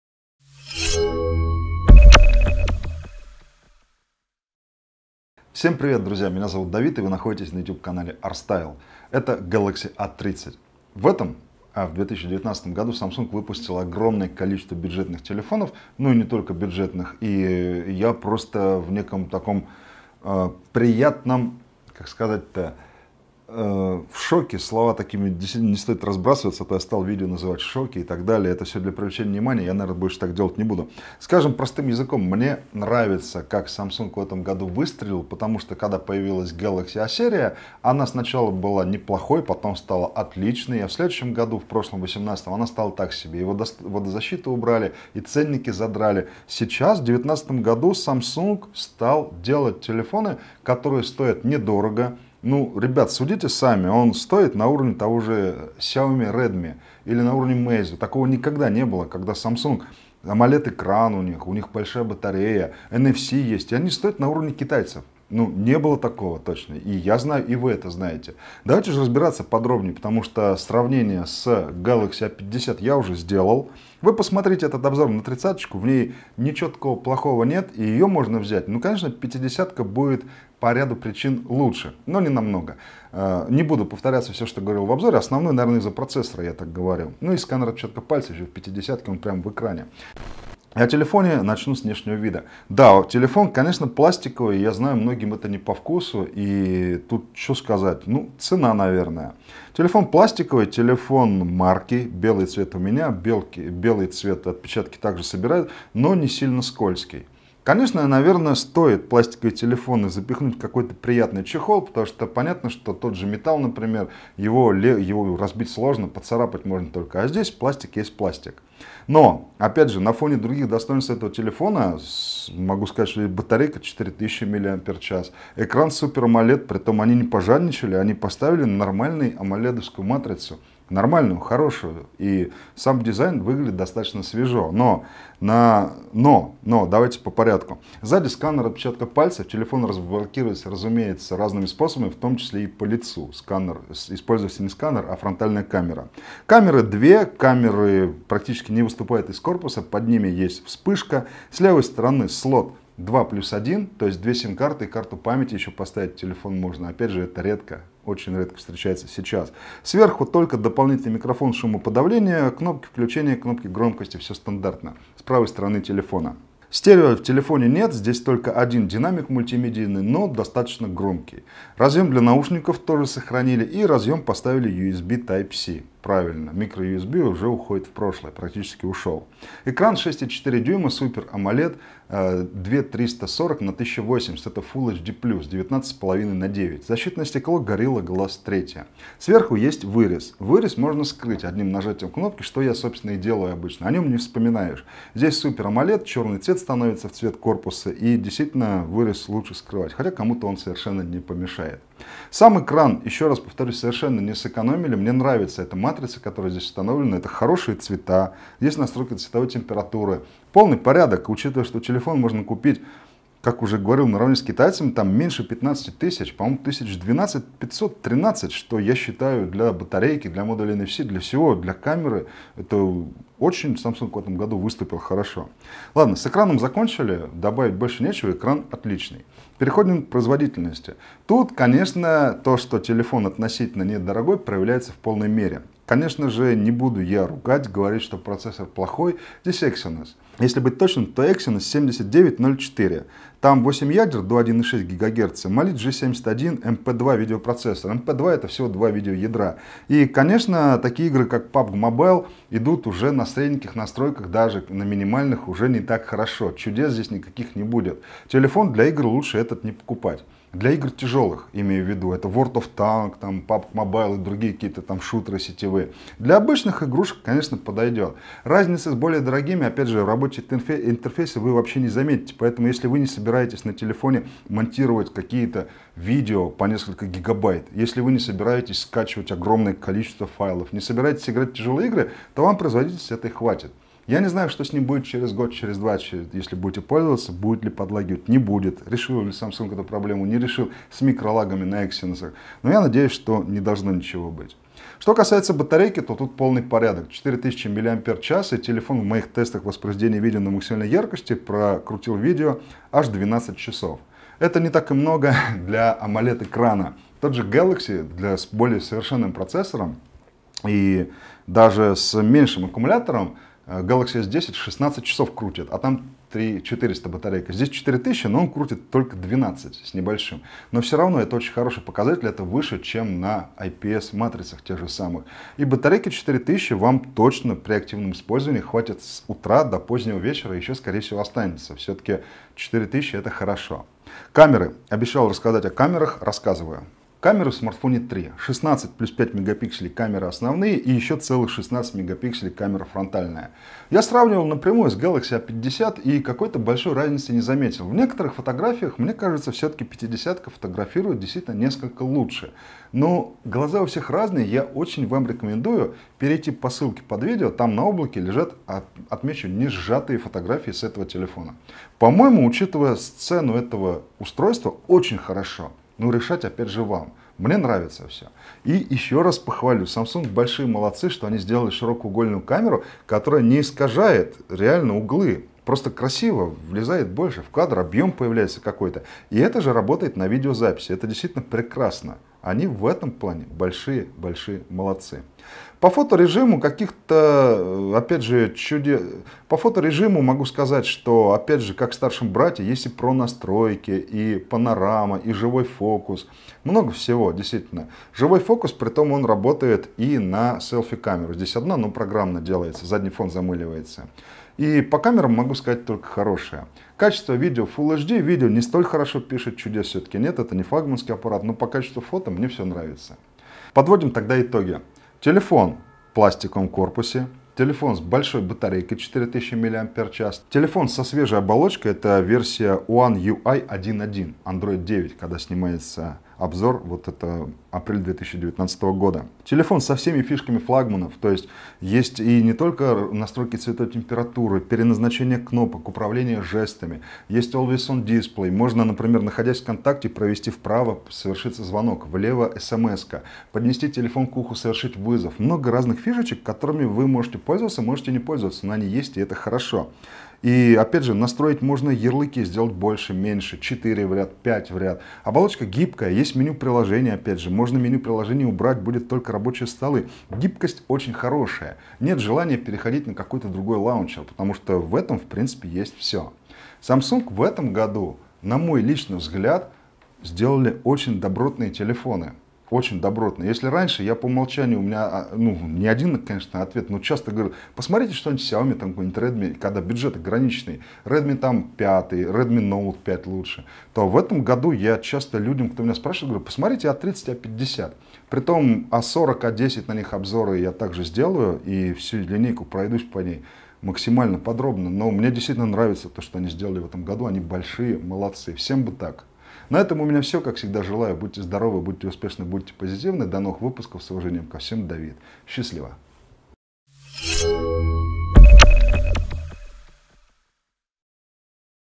Когда Samsung прозрел и выдал ЭТО! Galaxy A30 (2019) /от Арстайл /
Зеркало канала, абсолютно всё то же самое что на YouTube, на Дзен Новый Samsung Galaxy A30 - "огонь"! Amoled экран, нет рамок, есть NFC и батарея 4000 мАч, широкоугольная камера, и все это по цене Xiaomi или Honor - но даже лучше! Тест автономности, игр, камер и всего - всего!